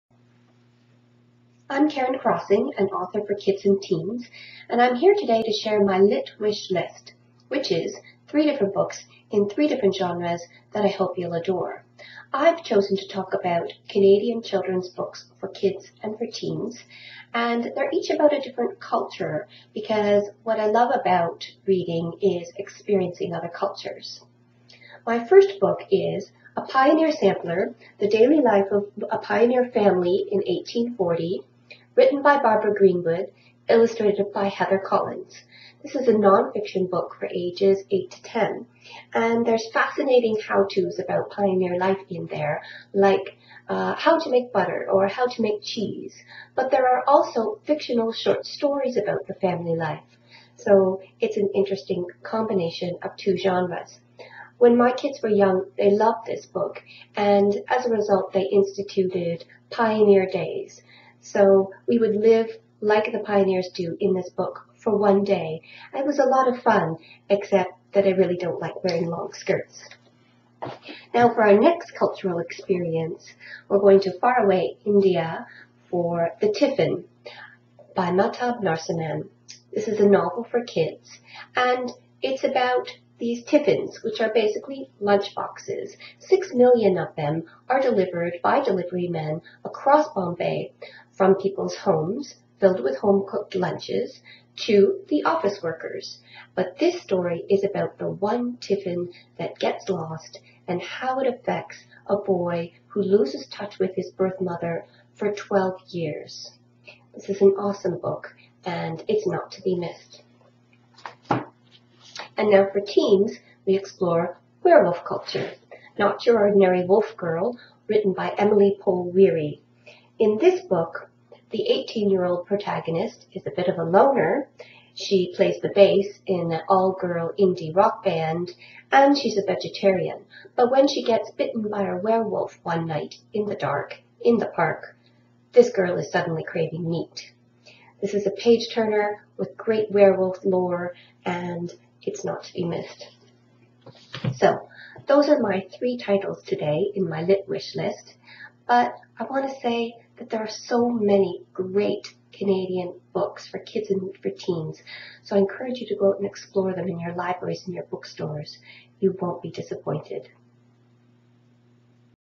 Lit Wish List 2013: Karen Krossing
For 49th Shelf, kids and teens author Karen Krossing recommends a few great books to give this holiday season.